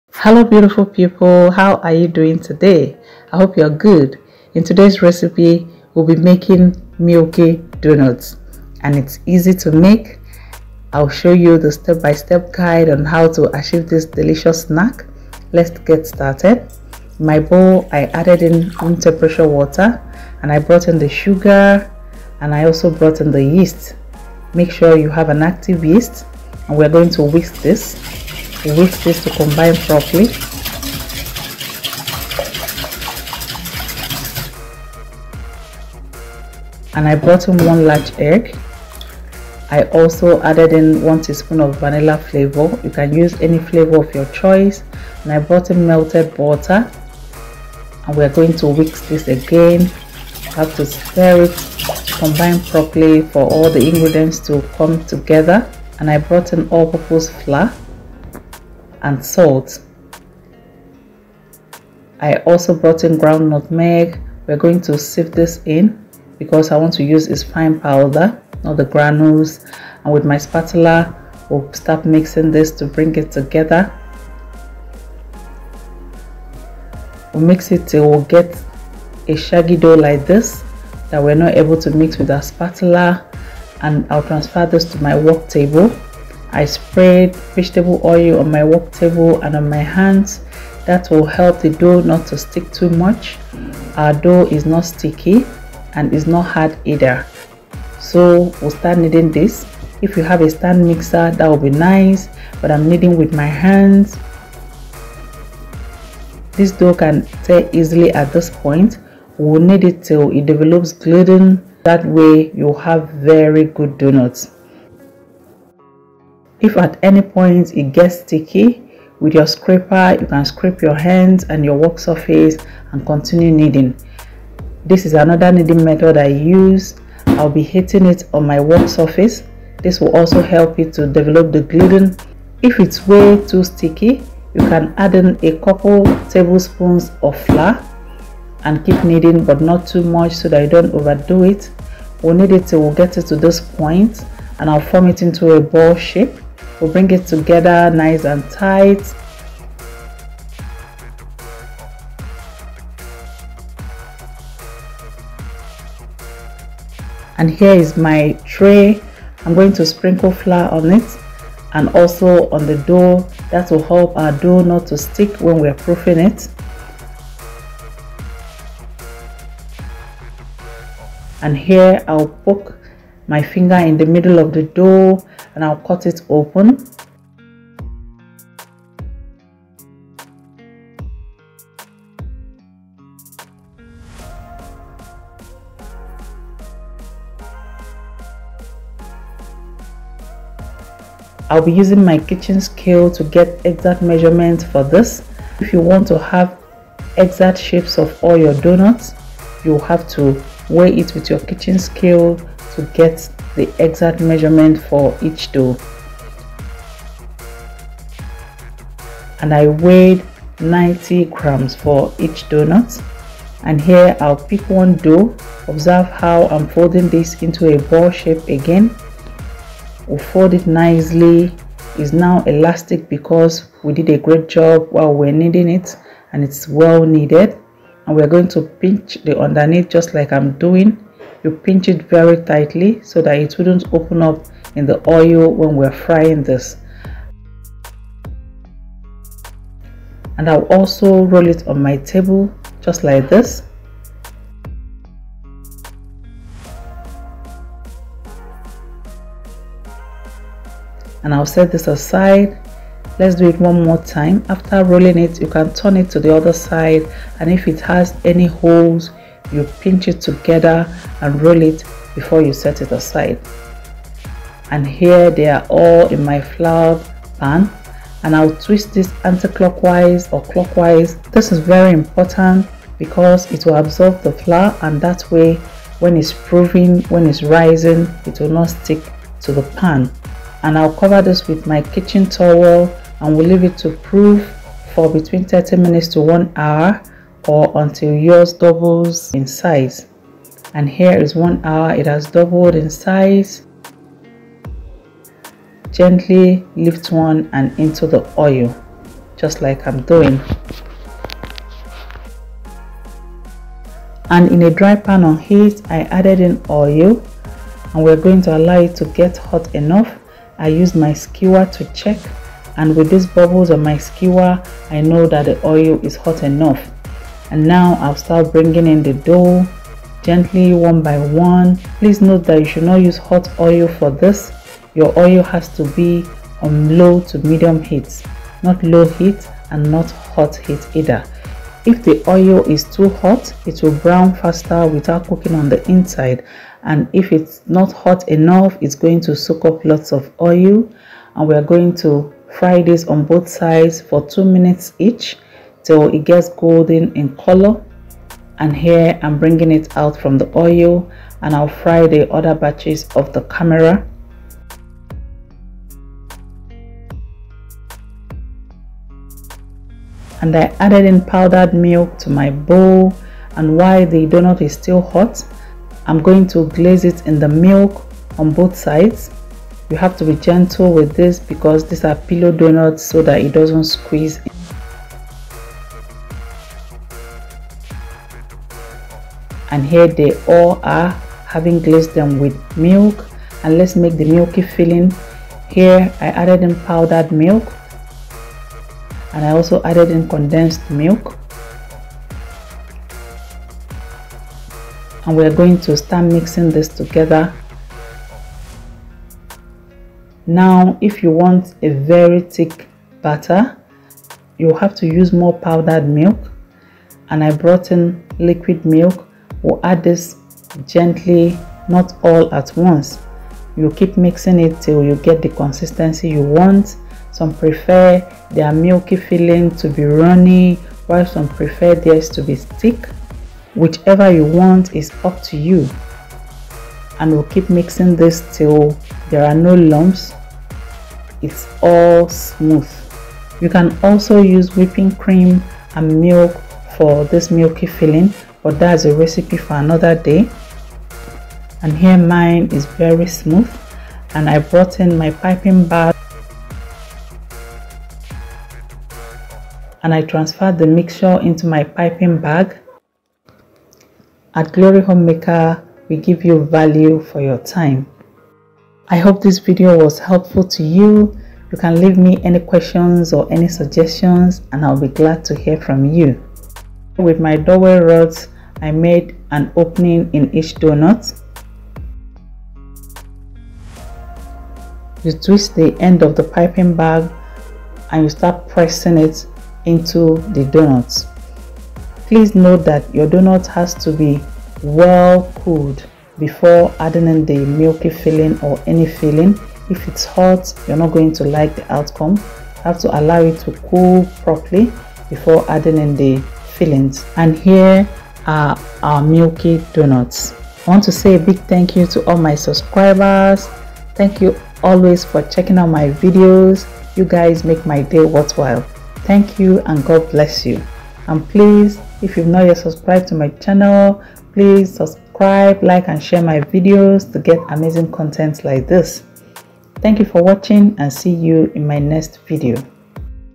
How To Make Milky Filled Doughnuts In Simple Steps + Making Milky Donuts Filling Recipe
Hello beautiful people, we are making the trending milky filled donuts in simple step. Even as a beginner, this recipe has step by step easy to follow guide to making the best filled doughnuts with ease. I know you will learn a thing or more and leave me your questions or suggestions in the comment section and I will be glad to hear from you.